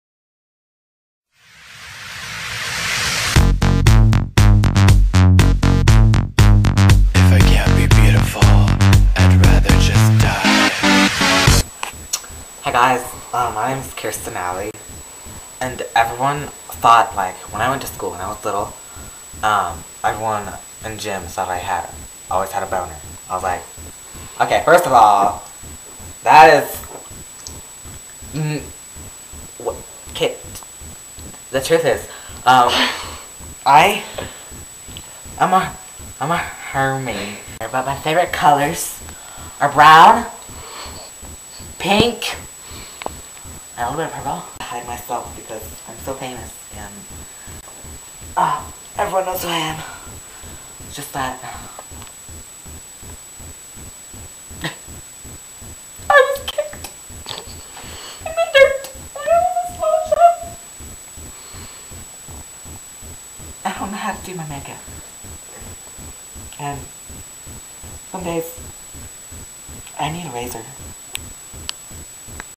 Introducing Kiersten Alley c:
This is Kiersten Alley. A very famous Russian Model. She has agreed to let me follow her around all week and record her adventures.